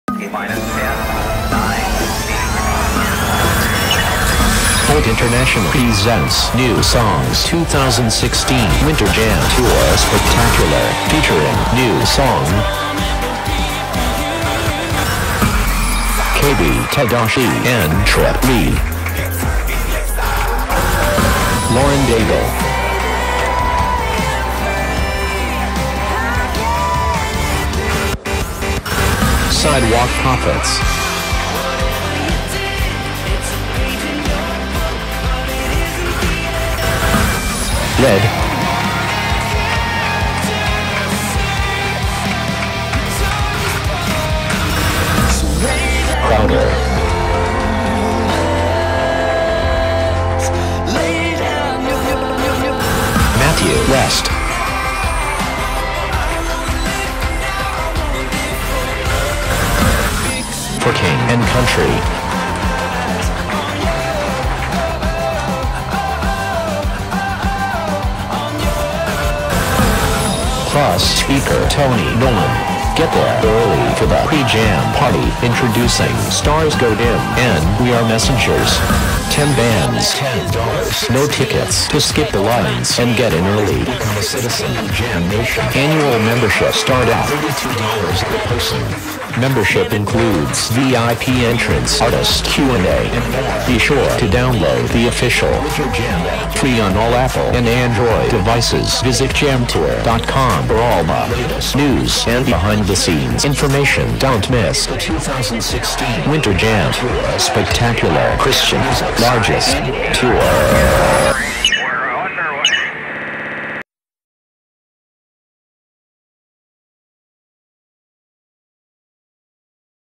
Winter Jam 2016 New Orleans
No ticket required- just $10 at the door!

Continuing an unprecedented reign as one of the world’s largest tours and Christian music’s biggest annual tour, the Winter Jam Tour Spectacular announced its highly-anticipated 2016 line-up earlier this evening at Nashville’s Sudekum Planetarium. The announcement, which included performances from several 2016 artists, was also live-streamed to viewers around the world via Blue Scout Media’s StageHop.com.

Artists appearing at the Smoothie King Center include: GRAMMY® Award-winning duo for KING & COUNTRY, GRAMMY®-nominated singer/songwriter Matthew West, Crowder, RED, Sidewalk Prophets, Lauren Daigle, NewSong, Trip Lee, KB, Stars Go Dim and We Are Messengers